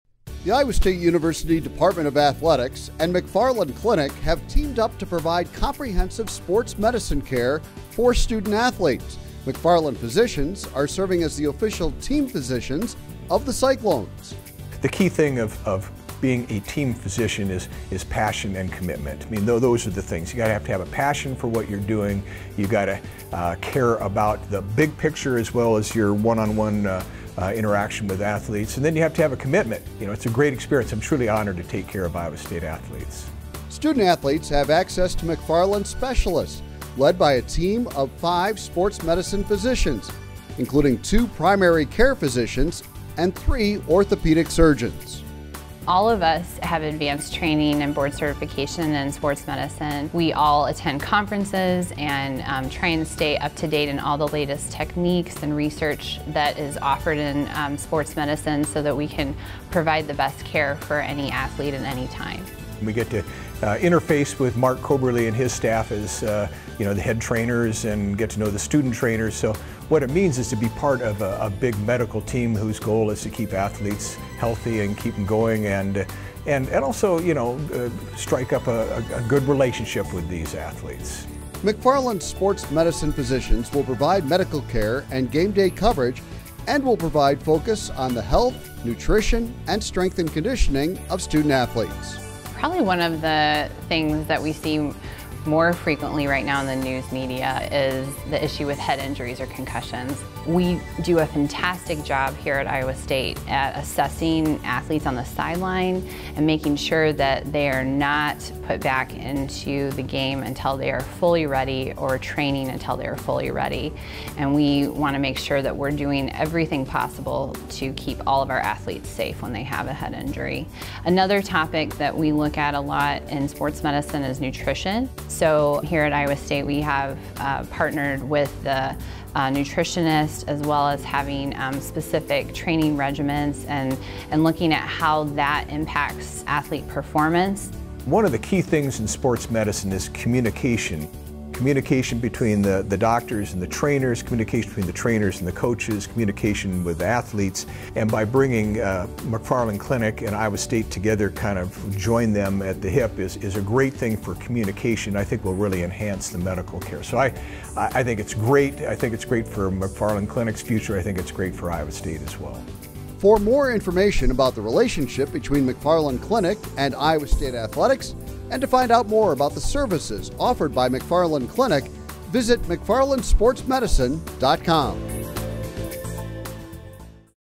Iowa State McFarland Sports Medicine | McFarland Clinic
The Iowa State University Department of Athletics and McFarland Clinic have teamed up to provide medical care to student athletes. The physicians of McFarland Clinic are the Official Team Physicians of the Iowa State Cyclones.